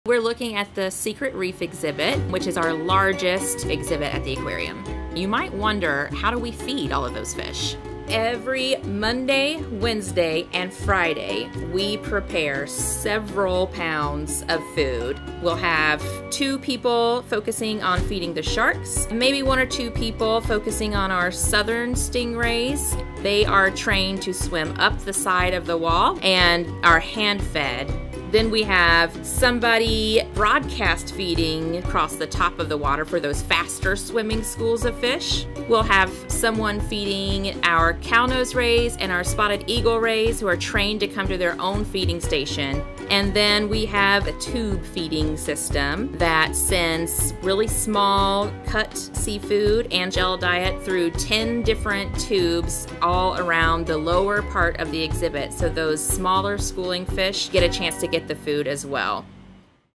Ask the Aquarium: How Do You Feed A 618,000-Gallon Exhibit?!?
The Secret Reef is the Aquarium's crown jewel, a 618,000-gallon recreation of the series of reefs in the Flower Garden Banks National Marine Sanctuary off the coast of Louisiana and Texas. This exhibit is home to thousands of animals, from colorful schooling reef fish and cruising Sand Tiger and Sandbar Sharks to a plucky Green Sea Turtle and graceful stingrays.

So how in the world do we feed such a diverse aquatic community? Well ... let's just say it's very much a team effort.